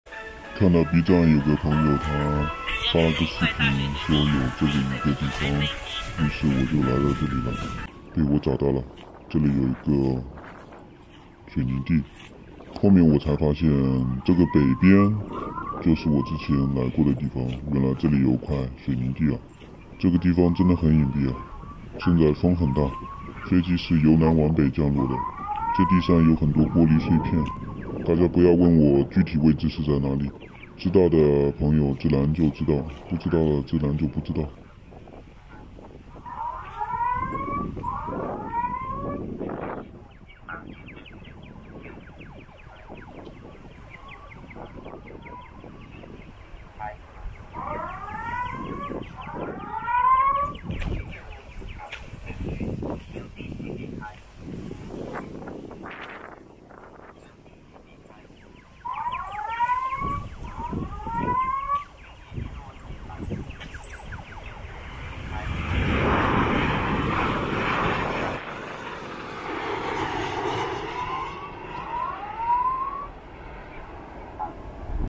找到一块大空地。这个地方拍飞机还可以。广州白云国际机场
①Blinkload翻墙机场
②快连VPN（LetsVPN）,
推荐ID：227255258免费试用